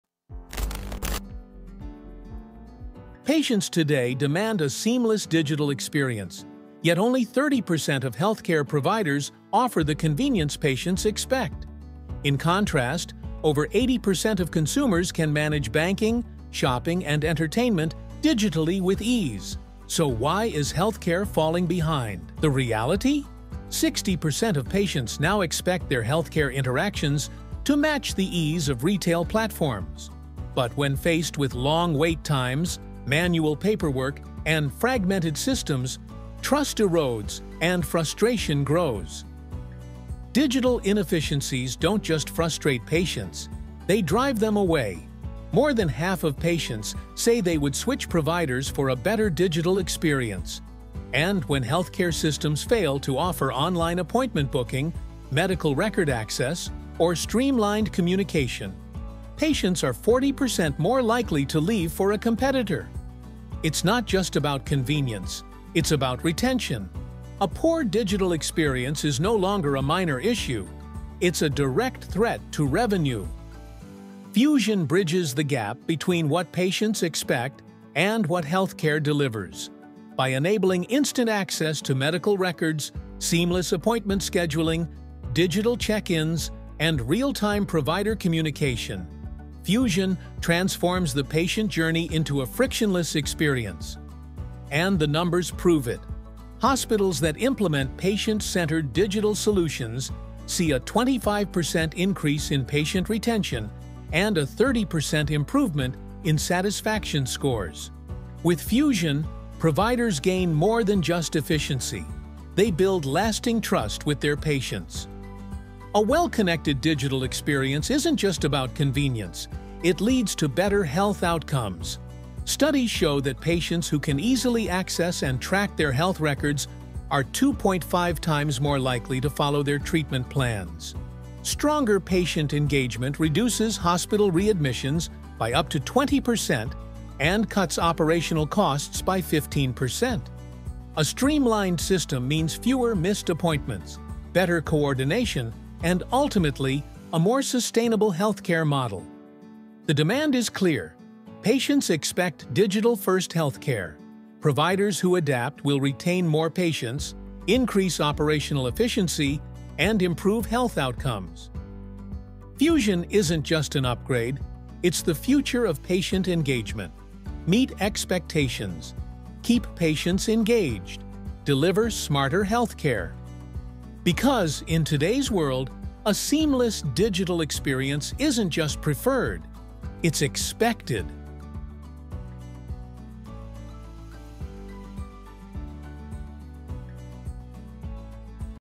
60% of Patients Expect Digital Healthcare - Are You Ready? | Helixbeat
Most healthcare providers are falling behind in offering the seamless digital experience patients expect—like easy appointment booking, record access, and communication. Over 60% of patients want healthcare to feel as convenient as online shopping or banking, and more than half would switch providers for better digital experiences.
Fusion solves this by offering:
• Instant access to medical records
• Seamless appointment scheduling
• Digital check-ins
• Real-time provider communication
This results in:
• 25% increase in patient retention
• 30% improvement in satisfaction scores
• 20% fewer readmissions
• 15% drop in operational costs
In short, Fusion bridges the gap between patient expectations and provider capabilities, leading to higher retention, better outcomes, and smarter, more efficient healthcare.